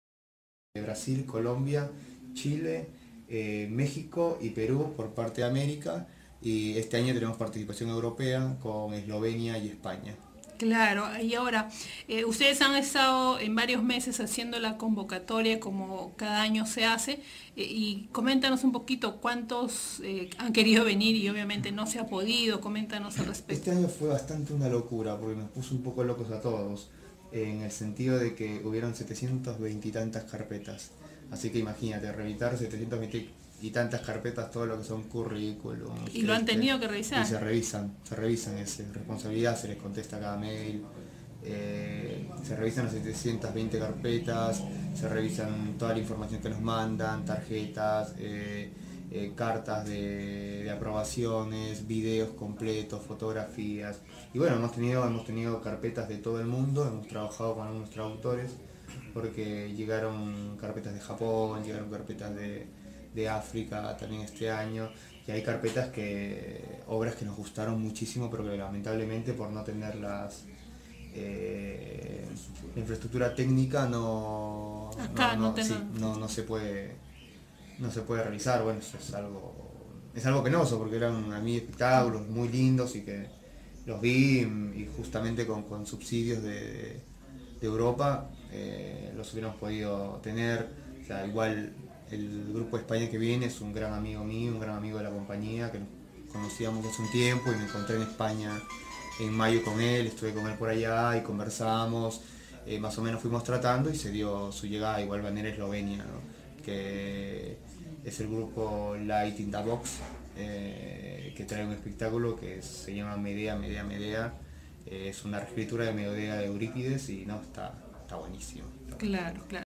CHANCAY: Novedades del octavo FESTEPE 2016
13 enero 2016.- Julio Martín Ortiz de la Asociación Cultural Muñecos de Madera informa las novedades para el octavo Festival de teatro y performance 2016 en Chancay.